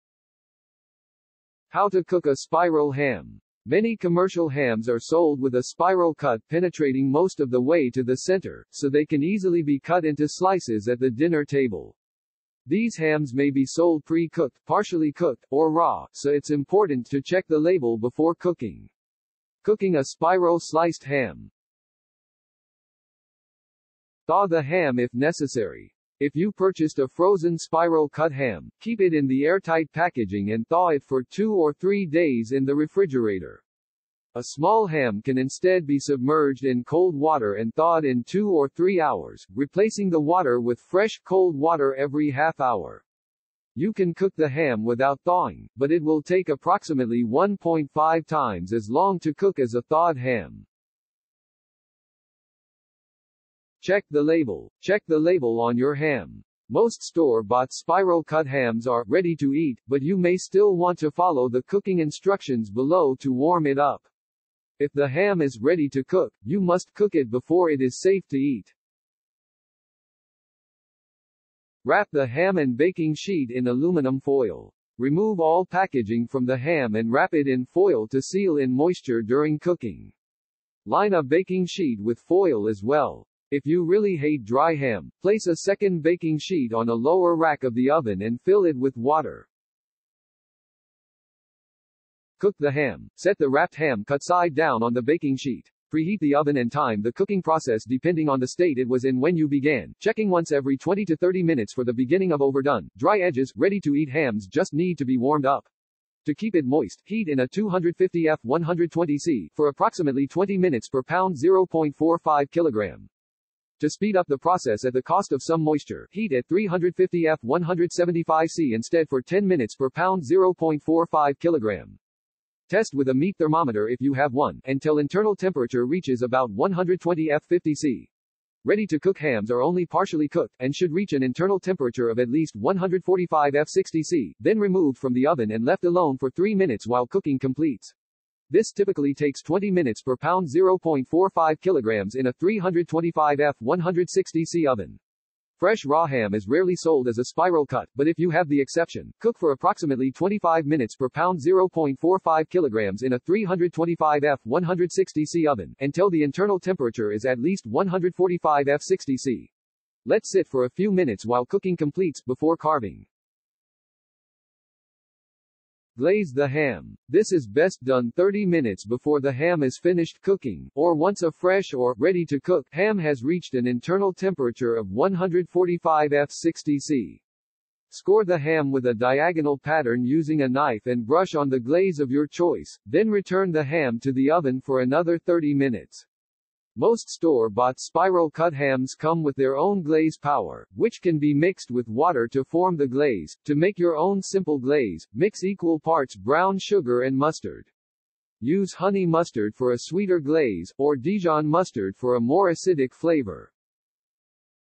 Creative Commons | How to Cook a Spiral Ham
How to Cook a Spiral Ham

Many commercial hams are sold with a spiral cut penetrating most of the way to the center, so they can easily be cut into slices at the dinner table. These hams may be sold pre-cooked, partially cooked, or raw, so its important to check the label before cooking.

Cooking a Spiral Sliced Ham Thaw the ham if necessary.

 If you purchased a frozen spiral-cut ham, keep it in the airtight packaging and thaw it for two or three days in the refrigerator. A small ham can instead be submerged in cold water and thawed in two or three hours, replacing the water with fresh, cold water every half hour.  You can cook the ham without thawing, but it will take approximately 1.5 times as long to cook as a thawed ham. Check the label.

 Check the label on your ham. Most store-bought spiral-cut hams are "ready to eat," but you may still want to follow the cooking instructions below to warm it up. If the ham is "ready to cook," you must cook it before it is safe to eat. Wrap the ham and baking sheet in aluminum foil.

 Remove all packaging from the ham and wrap it in foil to seal in moisture during cooking. Line a baking sheet with foil as well.  If you really hate dry ham, place a second baking sheet on a lower rack of the oven and fill it with water. Cook the ham.

 Set the wrapped ham cut-side down on the baking sheet. Preheat the oven and time the cooking process depending on the state it was in when you began, checking once every 20–30 minutes for the beginning of overdone, dry edges:   Ready to eat hams just need to be warmed up. To keep it moist, heat in a 250ºF 120ºC, for approximately 20 minutes per pound 0.45 kg. To speed up the process at the cost of some moisture, heat at 350ºF 175ºC instead for 10 minutes per pound 0.45 kg. Test with a meat thermometer if you have one, until internal temperature reaches about 120ºF 50ºC. Ready to cook hams are only partially cooked, and should reach an internal temperature of at least 14 Many commercial hams are sold with a spiral cut penetrating most of the way to the center, so they can easily be cut into slices at the dinner table. These hams may be sold pre-cooked, partially cooked, or raw, so its important to check the label before cooking.

Image Attributions